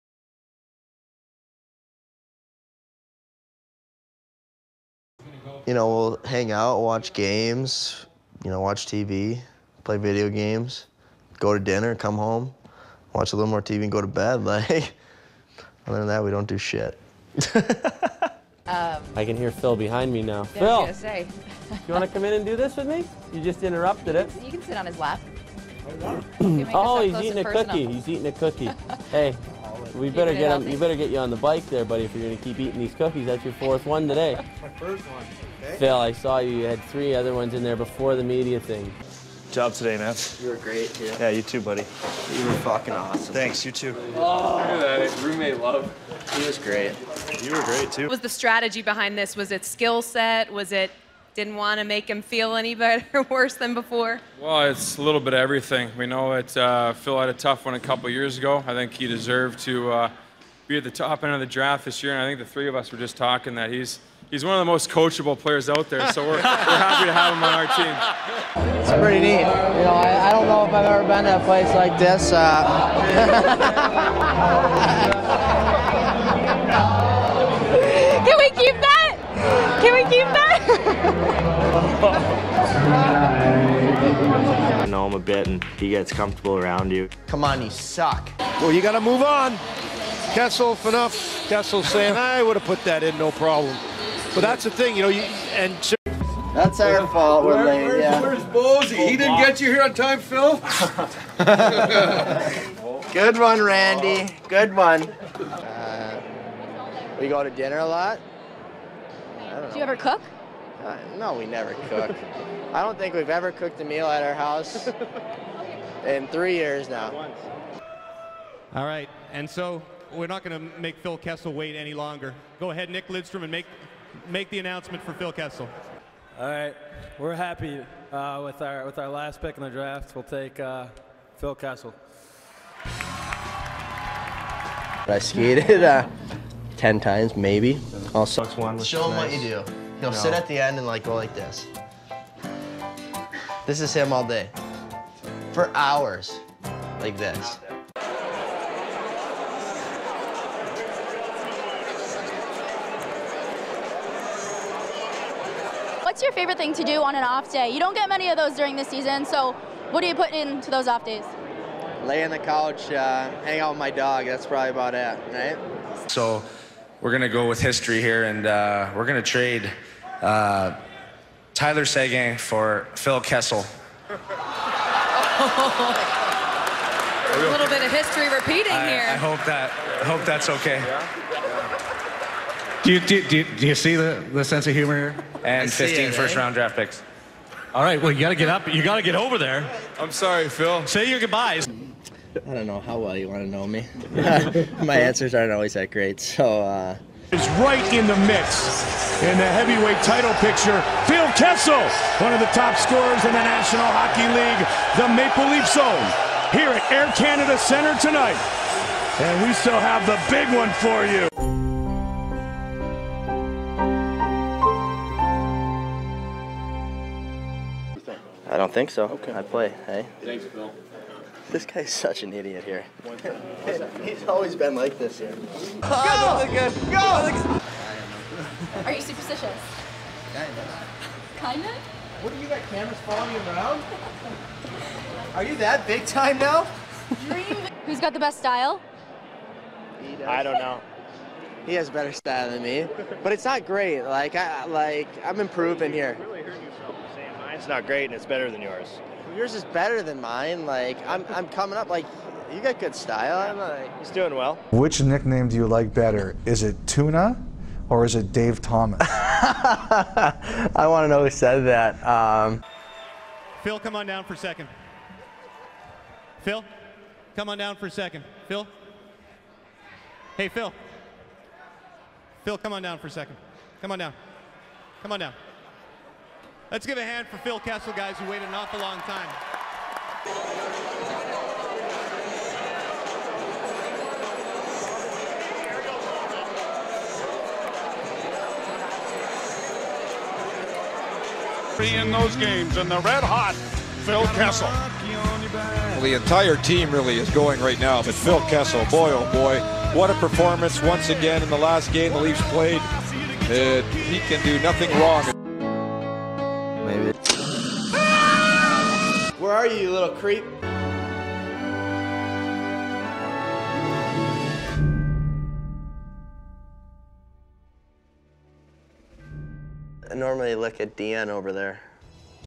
Phil Kessel - Funny Moments [HD]
Compilation of the funniest moments and bloopers from Phil Kessel (Pittsburgh Penguins).

I don´t own any of this videos.